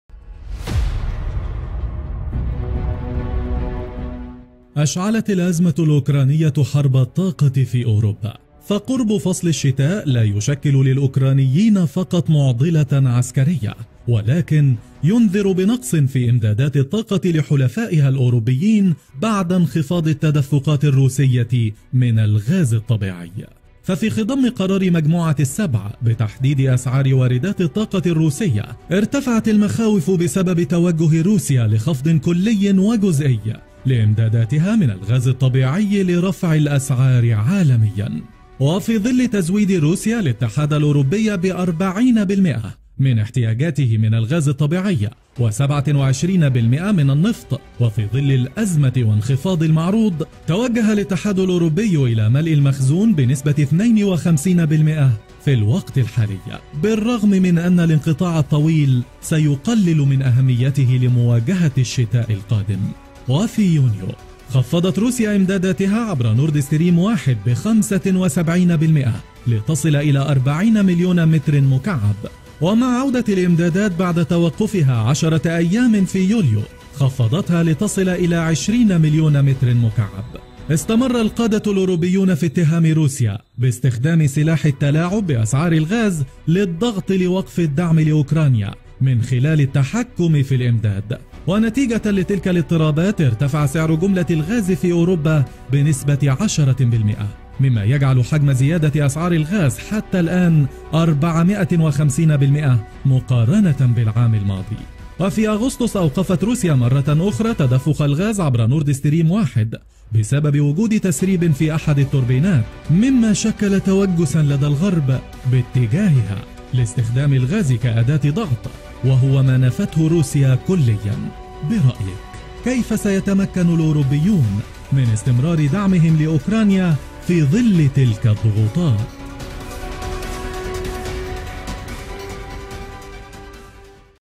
حرب الغاز في أوروبا
نشر مركز المعلومات ودعم اتخاذ القرار فيديو جديدا بعنوان "حرب الغاز في أوروبا" يسلط فيه الضوء على أبعاد أزمة الطاقة في روسيا؛ فقد أشعلت الأزمة الأوكرانية حرب الطاقة في أوروبا؛ فقرب فصل الشتاء لا يشكل للأوكرانيين فقط معضلة عسكرية ولكن ينذر بنقص في إمدادات الطاقة لحلفائها الأوروبيين بعد انخفاض التدفقات الروسية من الغاز الطبيعي. ففي خضم قرار مجموعة السبع وضع حد أقصى لأسعار واردات الطاقة الروسية.. ارتفعت المخاوف بسبب توجه روسيا لخفض كلي وجزئي لإمداداتها من الغاز الطبيعي، لرفع الأسعار عالميًّا. وفي ظل تزويد روسيا الاتحاد
الأوروبي بـ40% من احتياجاته من الغاز الطبيعي و27% من النفط (400 مليار يورو سنويًّا قيمة مشتريات النفط والغاز الروسي من قبل أوروبا)، أكدت شركة "إيني الإيطالية" تلقيها 1/2 الكمية التي طلب…